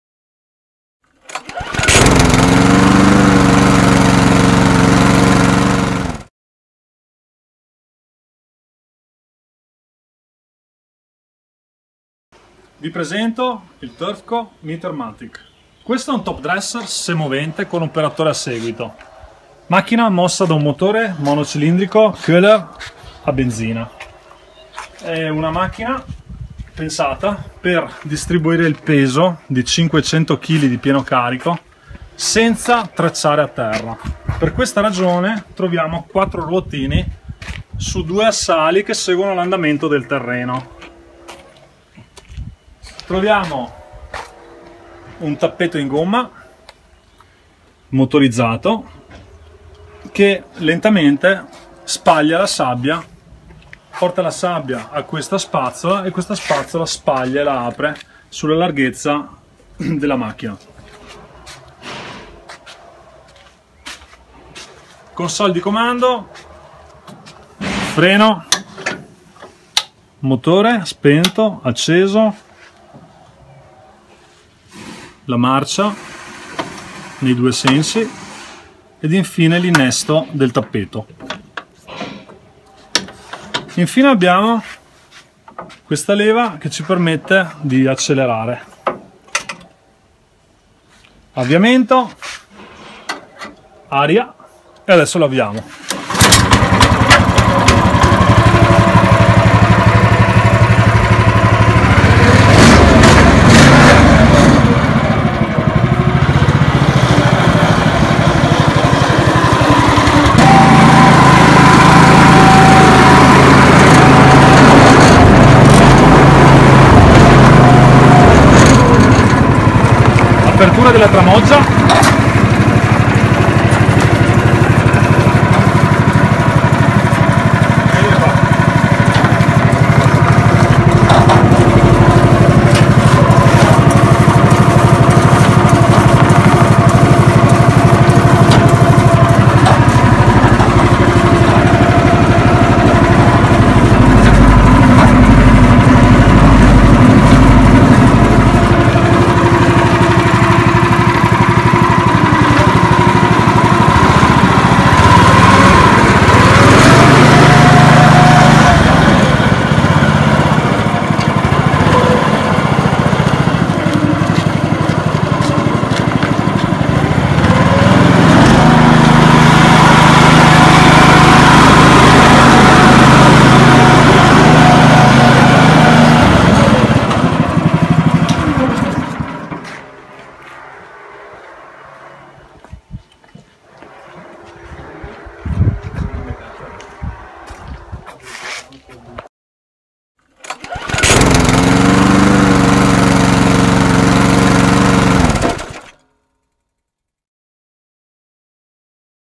TURFCO METE-O-MATIC - Top dresser/ spargisabbia
Prova dello spargisabbia top dresser Turfo METE-O-MATIC. Macchina adatta alla manutenzione sia di campi da golf ma pure di campi sportivi.

Per maggiori INFO e Preventivi:
Punto Vendita, Showroom e Assistenza tecnica.
Via Mason 5, Nanto (Vi)
Alessandro Bonato